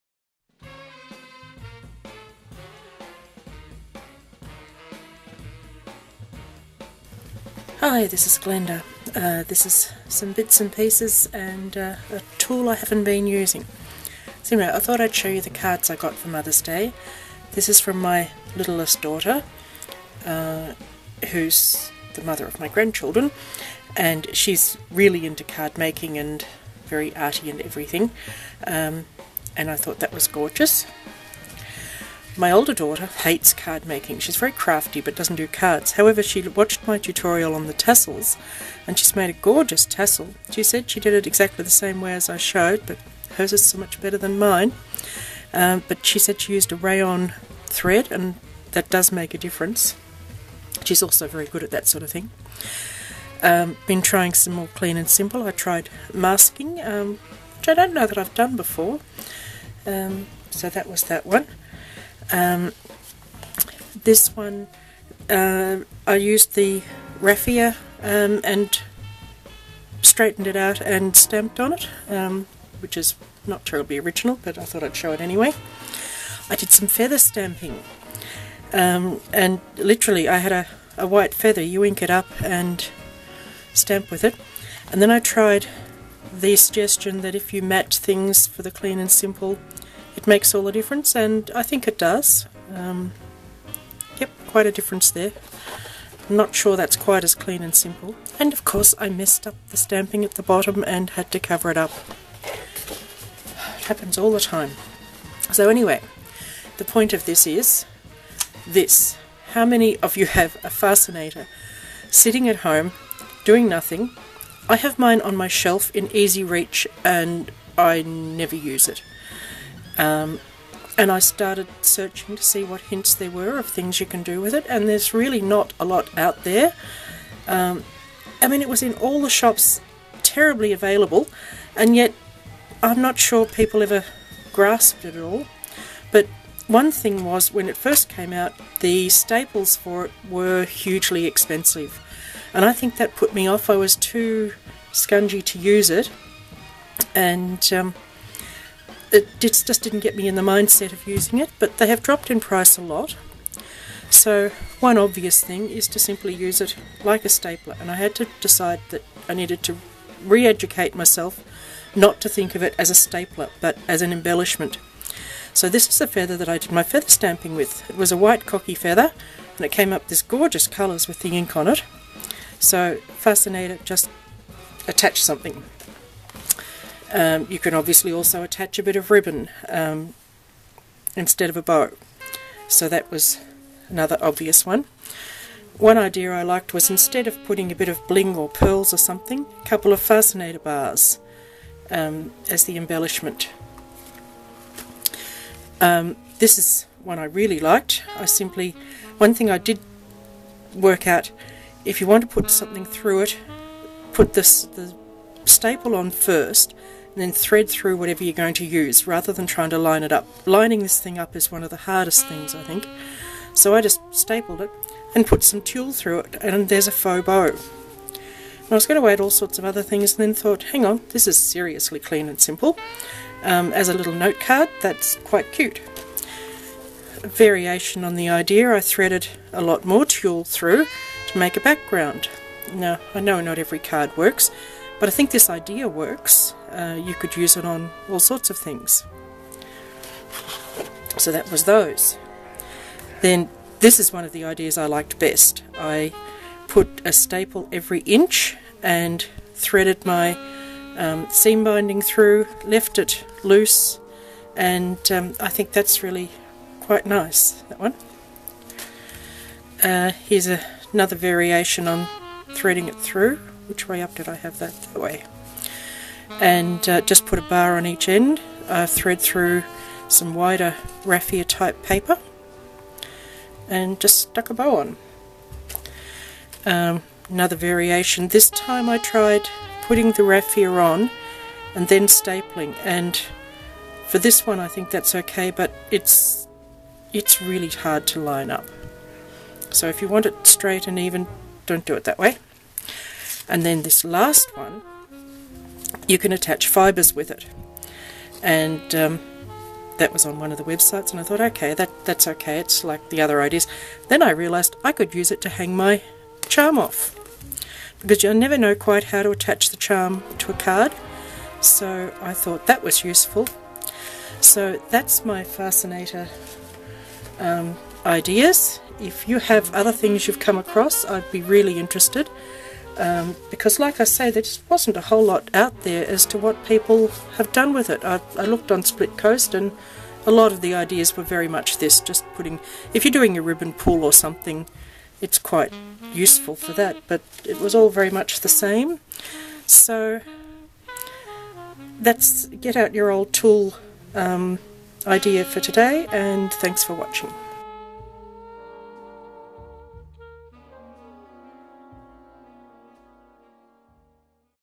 Bits & pieces & The Fastenater
What I've been doing and some ideas for using the EKSuccess Fastenater staples